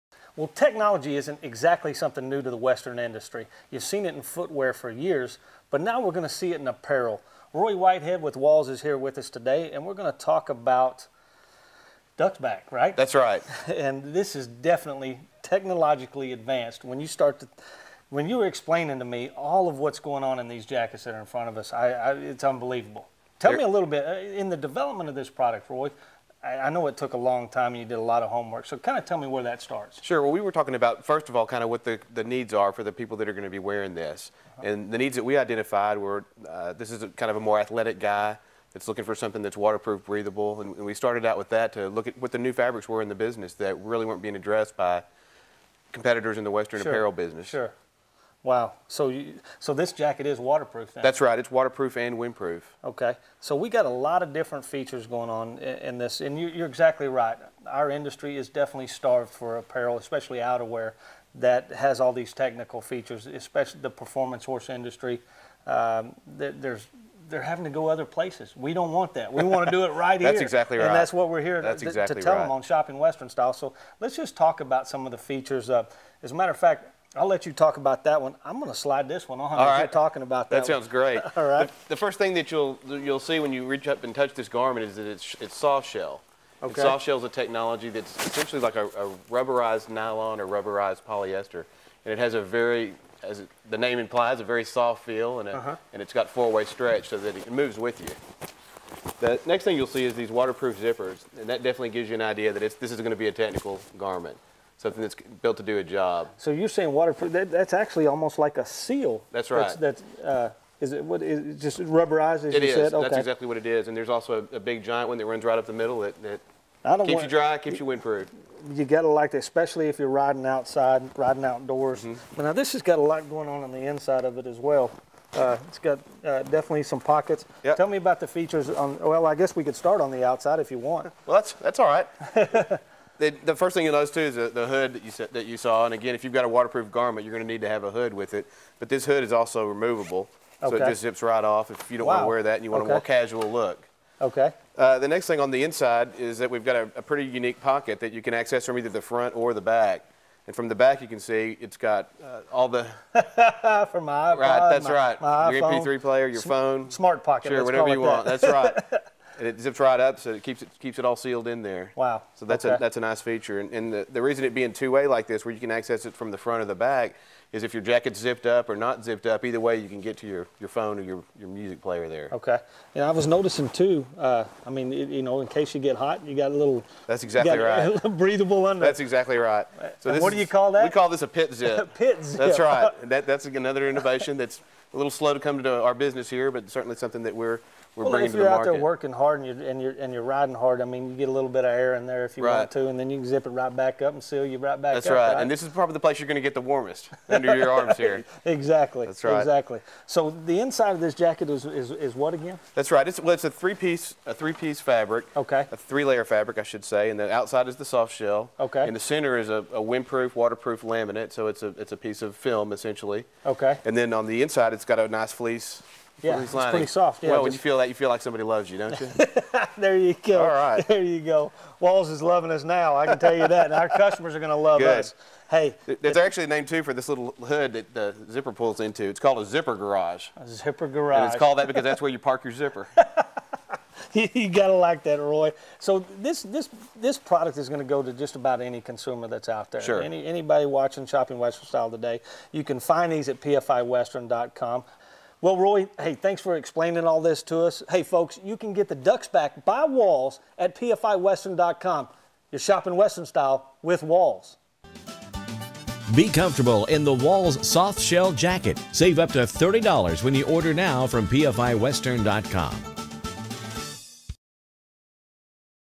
Walls Soft Shell Jackets
- Roy Whitehead from Walls is with us today to talk to us about the technologically advanced Walls Duxbak Soft Shell Jackets.
These waterproof and windproof jackets are perfect for riding or for everyday wear. Loaded with features, this jacket has waterproof zippers, a removable hood, fleece lining and a unique feature: a two-way pocket at the chest that you can access from the outside or inside of the jacket. Definitely a must for the athletic horseman or outdoorsman!